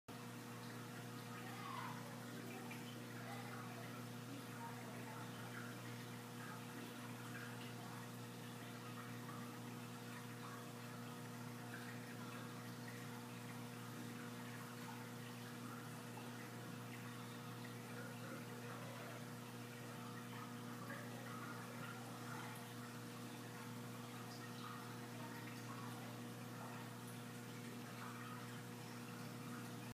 Clown Fish spawning
Cinnamon Clown Fish spawned solo, with no male in the tank.